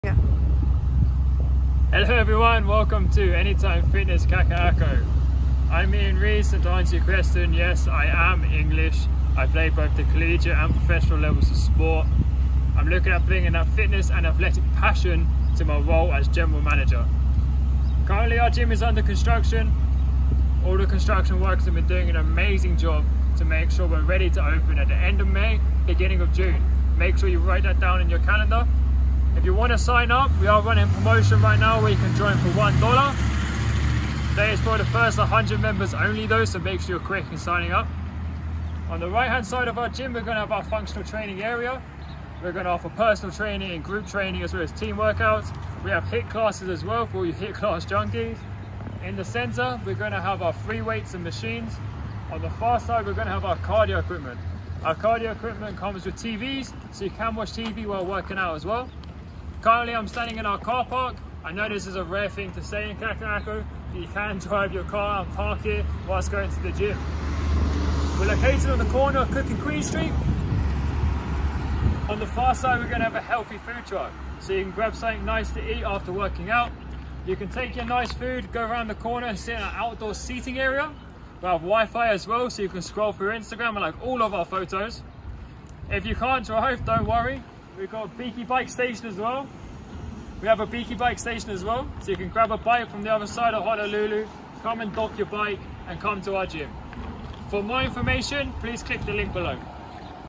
Anytime Fitness KAKA'AKO - JOIN FOR $1
GET READY KAKA’AKO

✅Healthy Food Truck
✅Biki Bike Station
✅Outside Seating Area
✅WiFi
✅Car Park
✅HIIT Classes
✅Brand New Equipment & MUCH MORE!
Despite the hardships that come with this time, we’re excited to be your local gym. Welcome to Anytime Fitness Kaka’ako‼️